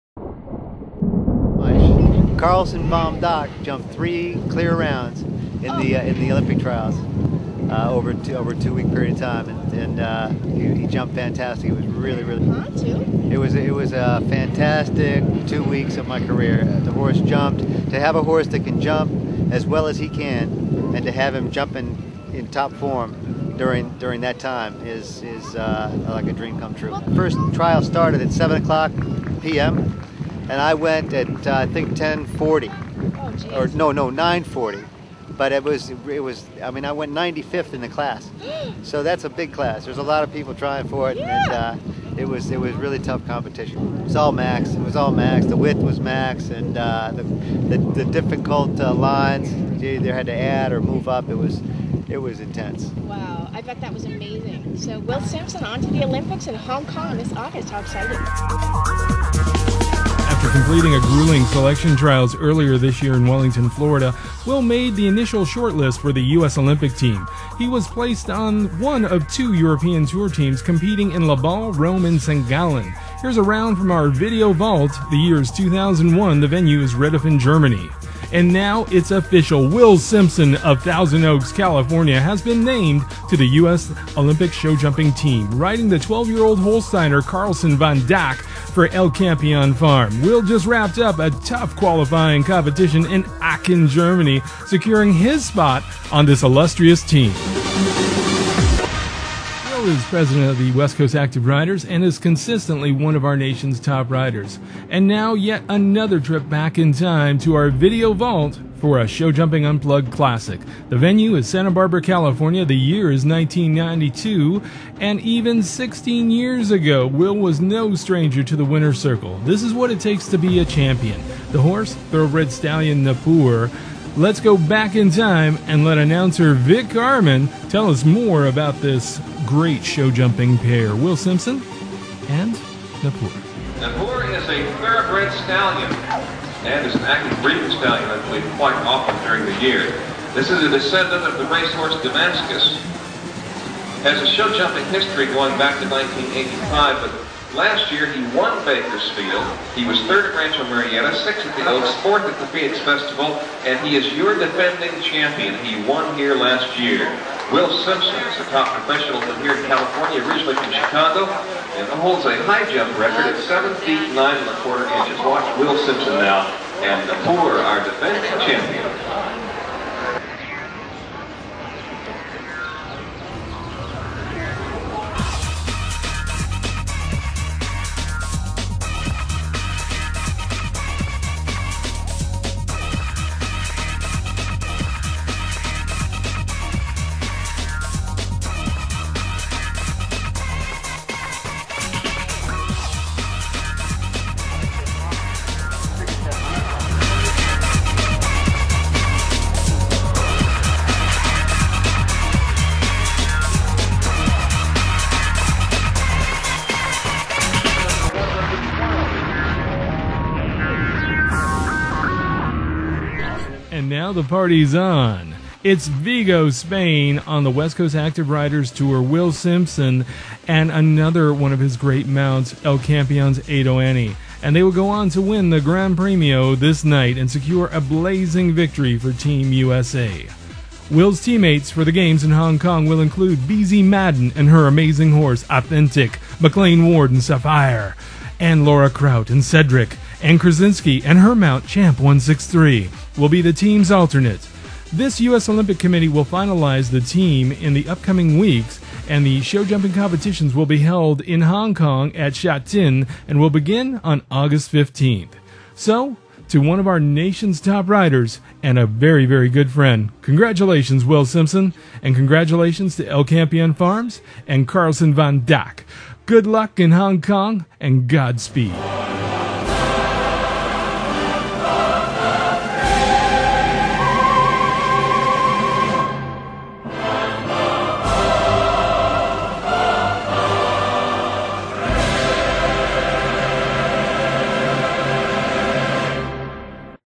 Showjumping Unplugged!TV Will Simpson Edition 2008
Thousand Oaks,CA- Join us as we catch up with Will Simpson and a blast from our video vault!